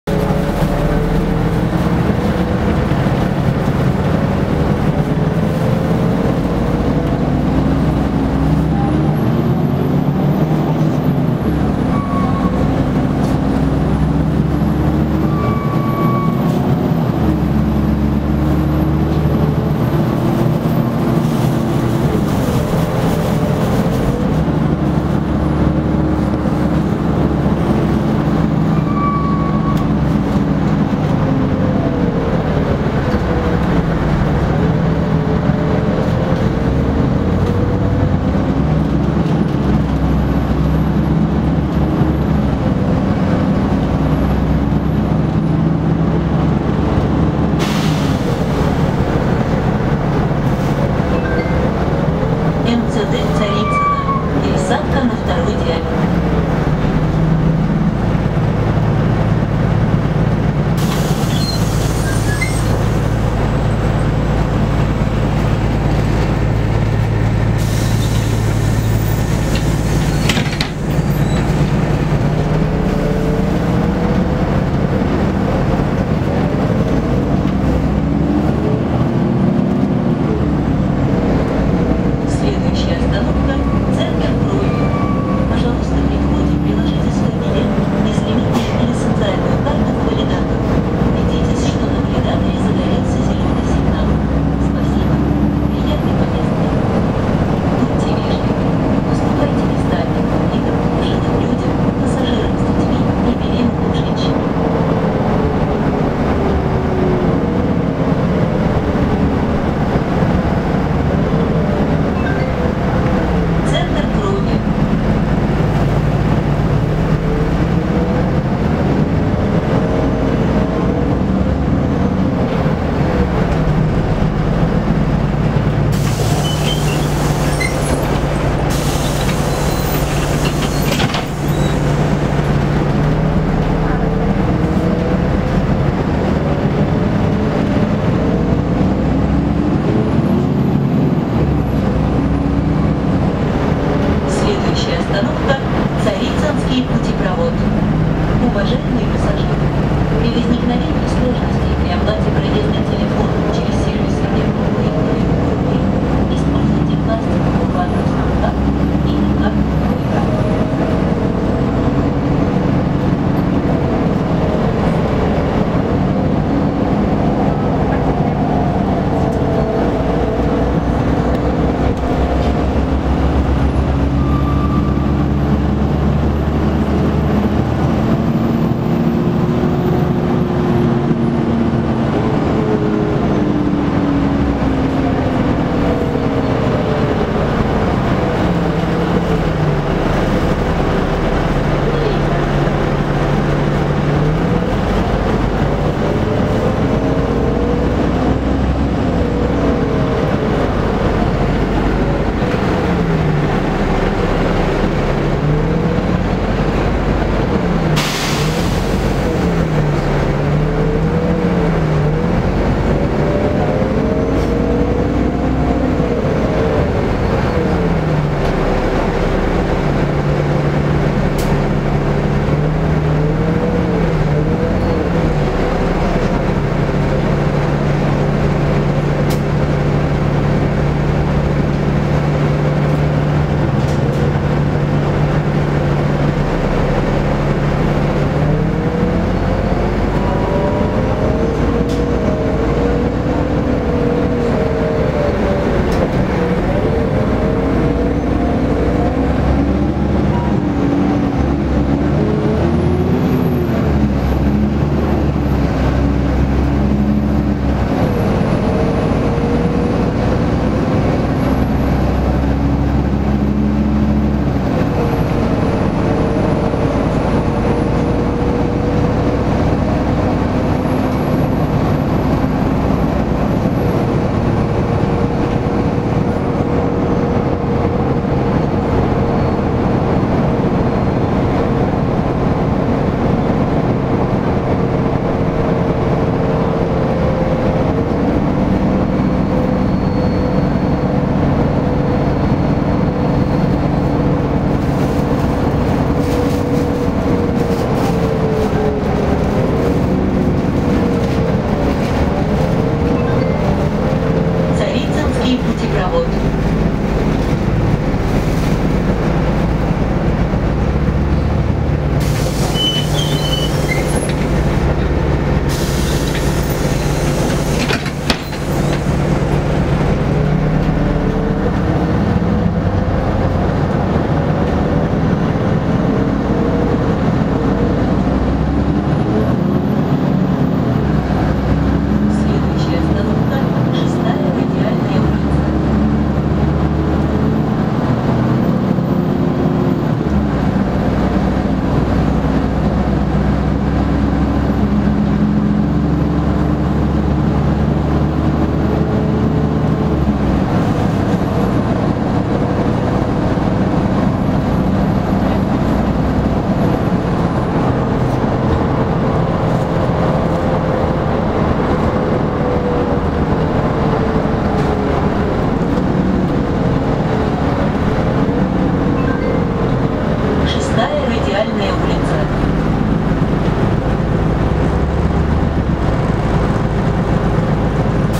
Из Царицыно - в Восточное Бирюлево и Загорье на магистральном маршруте М89
Отправляемся от станции метро "Царицыно" по Каспийской улице и выезжаем на Липецкую улицу и едем в Восточное Бирюлево и Загорье на автобусе ЛиАЗ-6213.65 по магистральному маршруту М89.